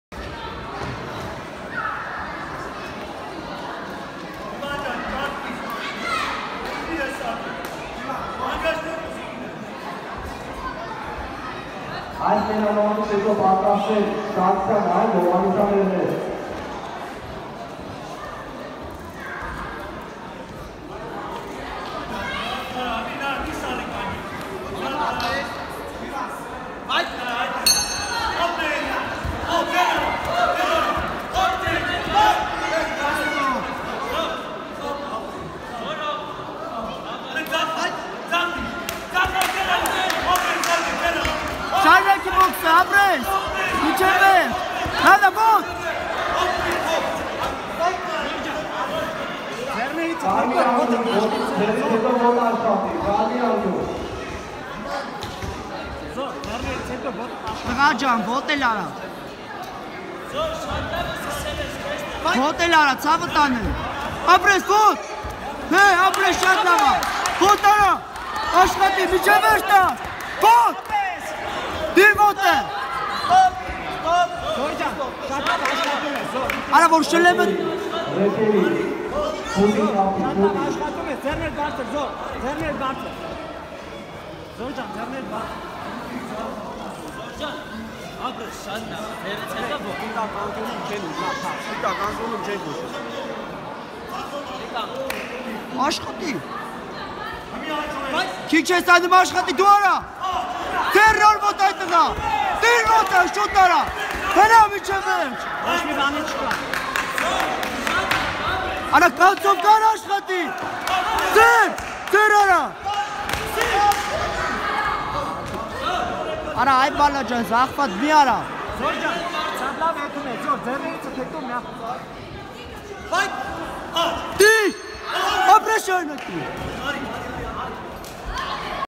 Ջիվան Բարոյան
13տ 37-կգ AK1 Armenian profesional K1 Federation N4 Turnament չեմպիոն 1 ռաւնդ